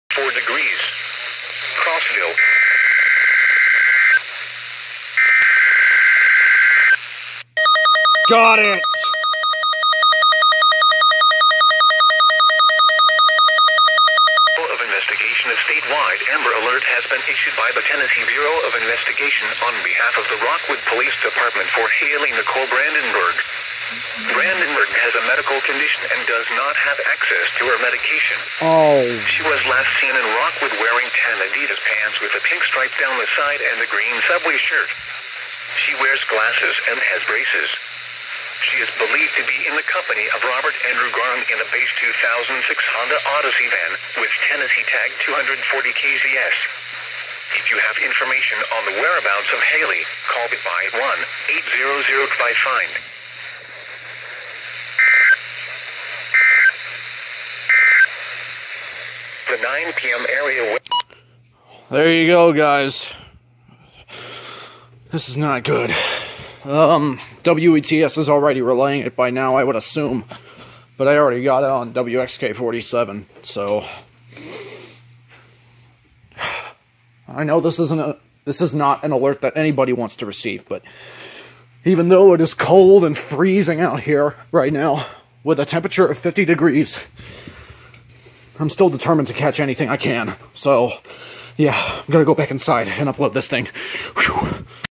Child Abduction Emergency on WXK47 - 11/8/18 (EAS #1,923)
The child has been found safe as of 11/14/18 in Henry County, Georgia.